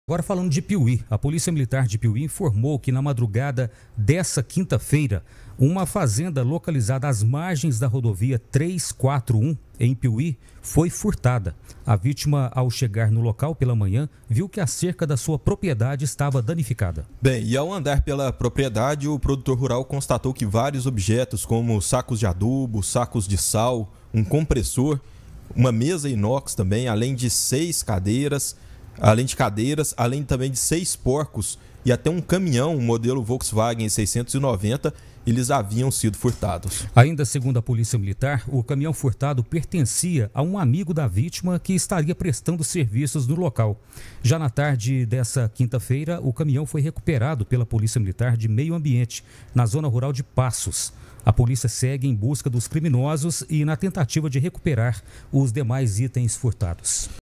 FAZENDA É FURTADA NA ZONA RURAL DE PIUMHI
| PIUMHI |

A POLÍCIA MILITAR DE PIUMHI INFORMOU QUE NA MADRUGADA DESTA QUINTA-FEIRA, UMA FAZENDA LOCALIZADA ÀS MARGENS DA RODOVIA MG-341, EM PIUMHI FOI FURTADA. SAIBA MAIS: